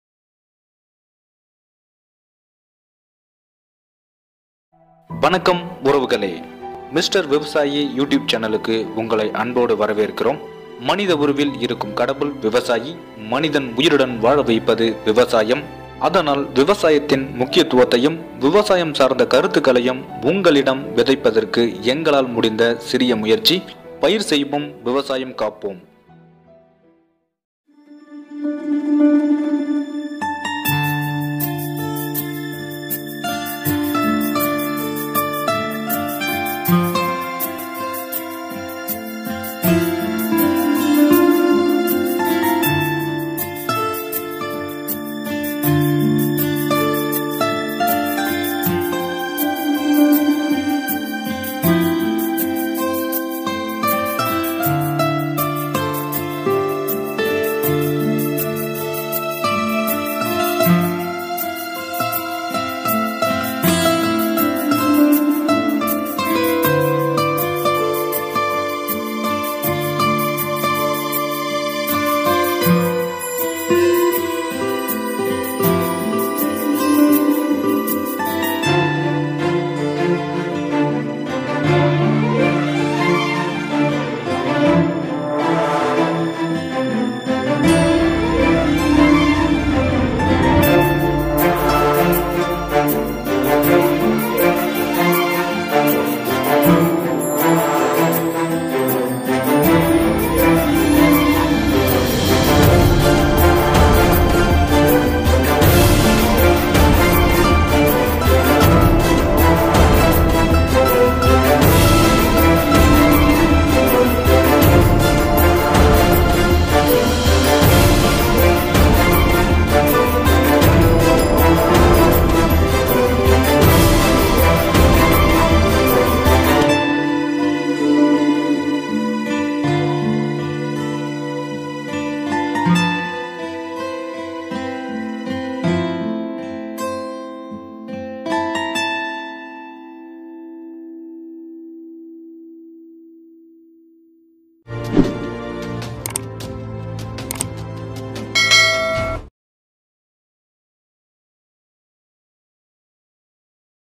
இலுப்பையின் மருத்துவ குணங்கள் | MR.VIVASAYEE
இலுப்பையின் மருத்துவ குணங்கள்

இலுப்பைப்பழம் நல்ல இனிப்புச் சுவை உடையது. மலச்சிக்கலைப் போக்கும் தன்மை கொண்டது. இலுப்பை விதையின் ஓட்டை நீக்கி உள்ளே உள்ள பருப்பை வதக்கி அரைத்து வீக்கங்களுக்கு கட்டினால் வீக்கம் விரைவில் குணமாகும். இலுப்பையின் விதையில் எடுக்கப்படும் எண்ணெய் உடலுக்கு வன்மையும் வனப்பையும் கொடுக்கும்.